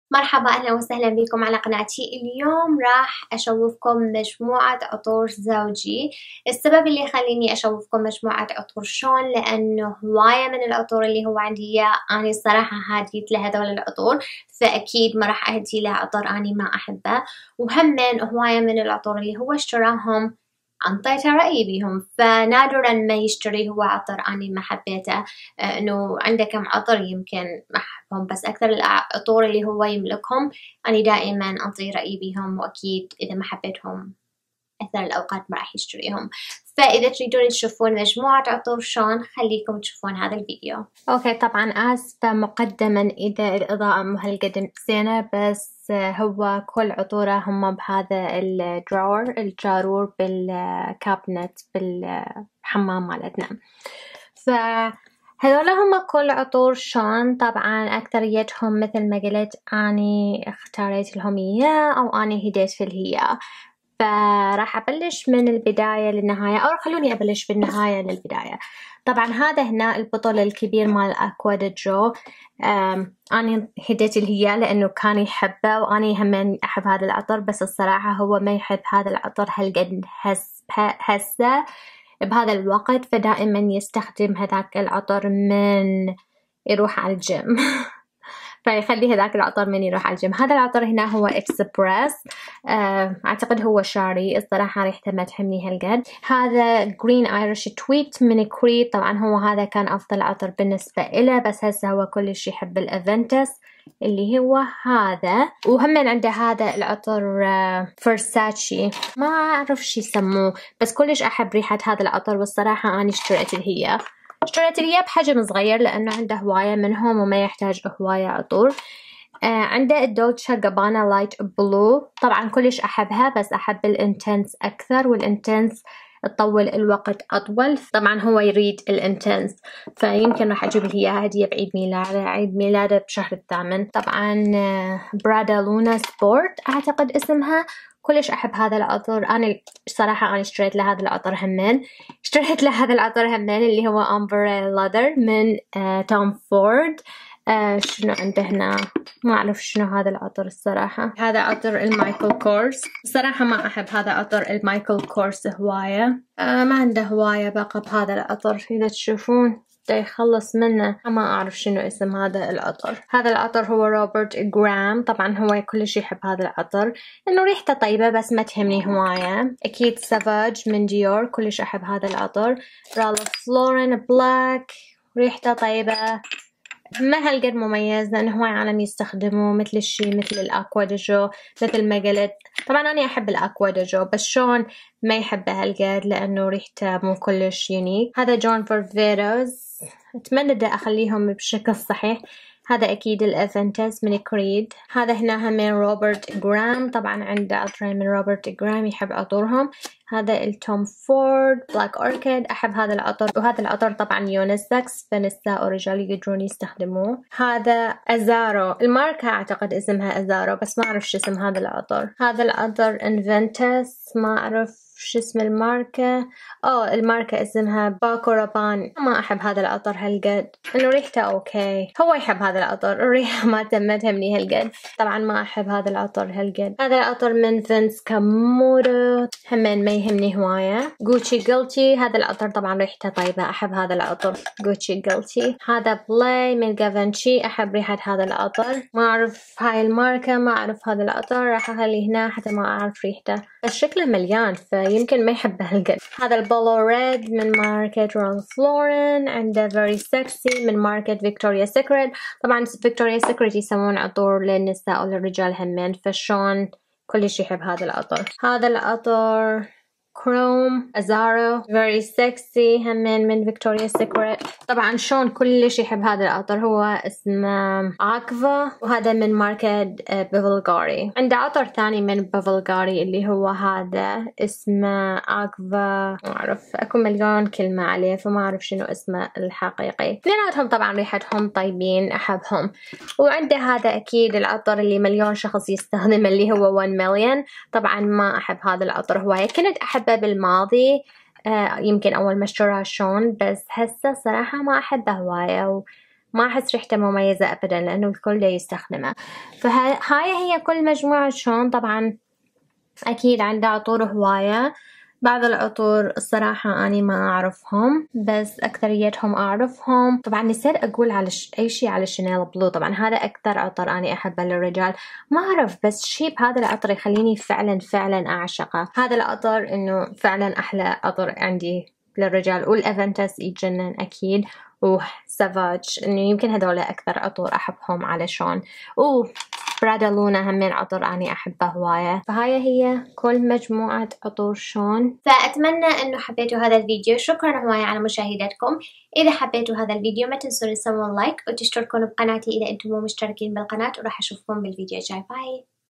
مجموعة عطور زوجي وتقييمي الهم | عطور رجالية
مرحبا! اهلًا و سهلاً بيكوم على قناتي. اليوم راح اشوفكم مجموعة عطور شون لان اكثر العطور الي يملكهم اعطيتا رأي بيهم ففكرت انو راح تحبون تشوفون مجموعة عطوره . شكرا على مشاهدتكم. أتمنى انو أتحبون هذا الفيديو. اذا حبيتو هذا الفيديو. أحبكوم